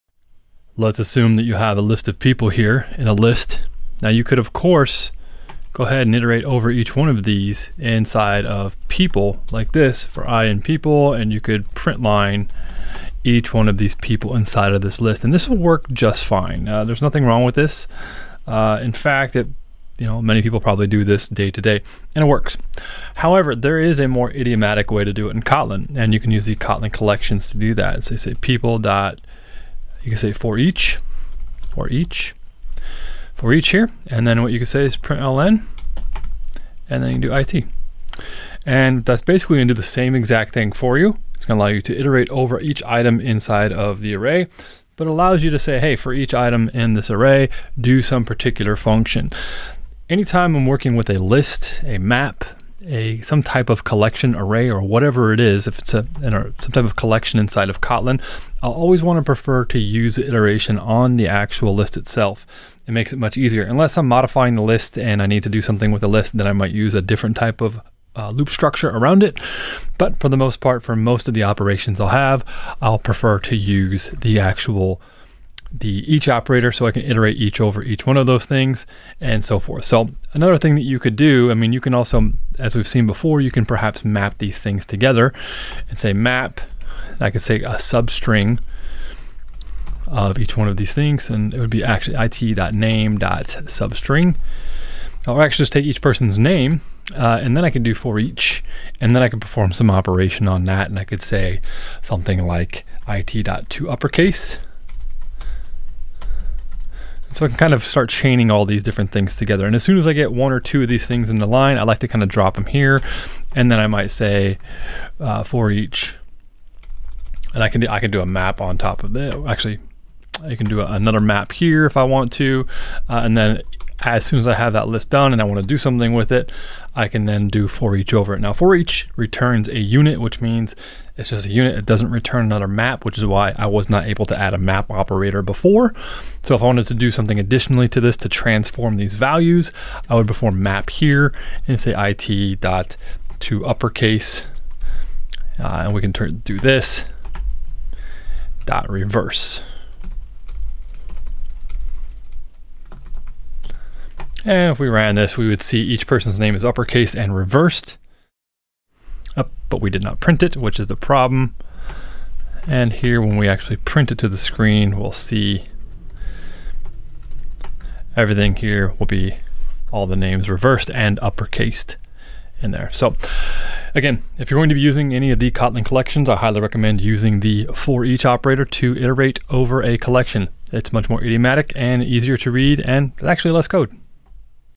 How to use forEach to Iterate over a Kotlin List or Collection
In this lesson, you'll learn how to use the forEach operator to loop over a collection in Kotlin.